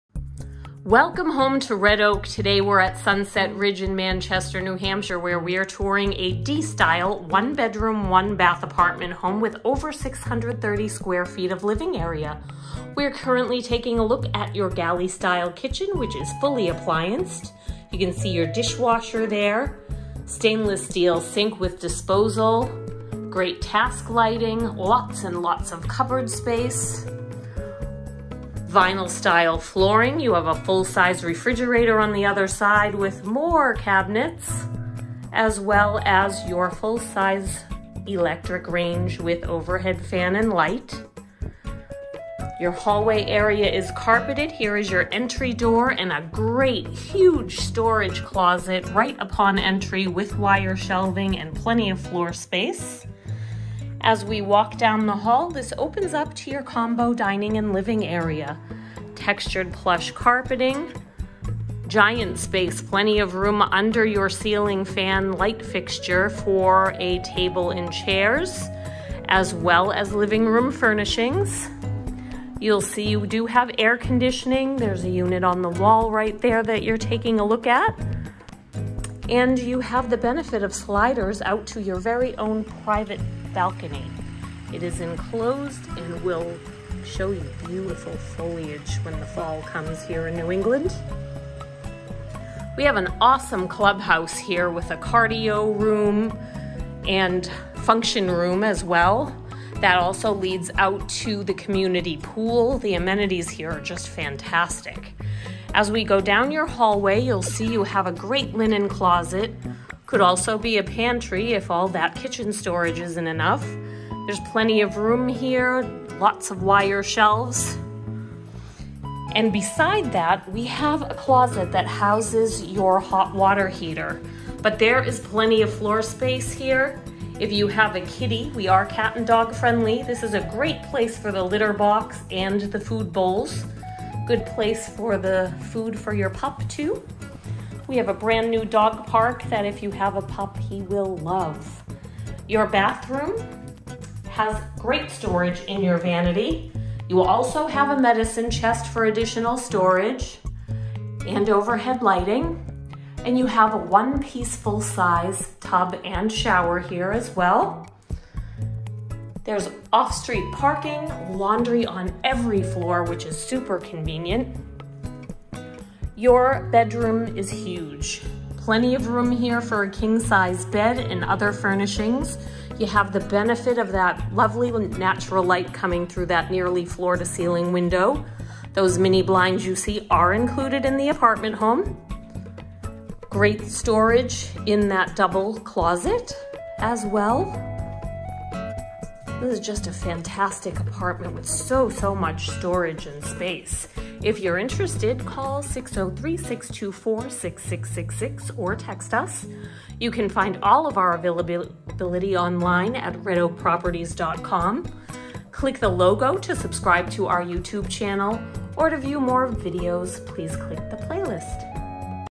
Sunset Ridge 119-204 - Video Tour a Manchester, NH 1-BR apt. - pet friendly w/great amenities.
Video Tour a 1-bedroom apartment at Sunset Ridge in Manchester, NH. This 2nd. floor non-smoking apartment features fantastic amenities: laundry on each floor, clubhouse with cardio and function rooms, community pool, playground, dog park and more. Pet friendly too!